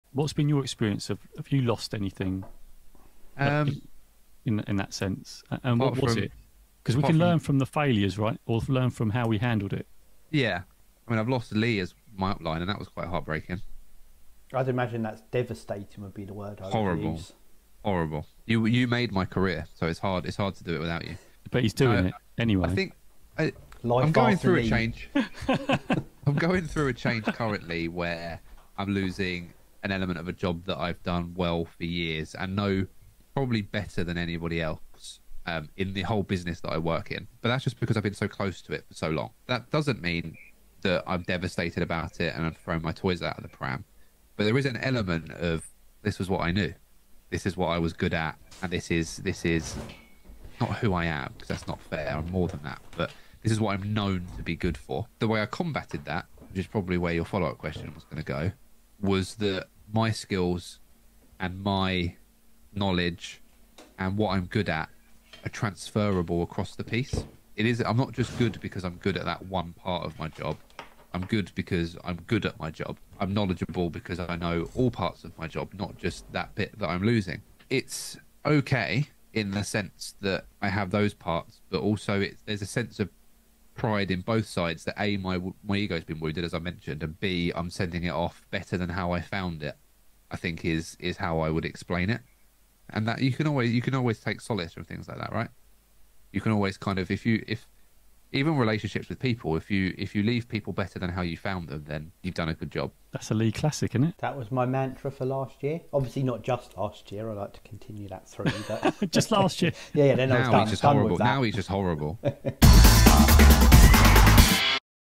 This is how you DEAL With FAILURE!
This is how you deal with failure.  In this clip Ryan explains what's happened to him and how we can all move through failure, we all chip in and we would love your view!

Much love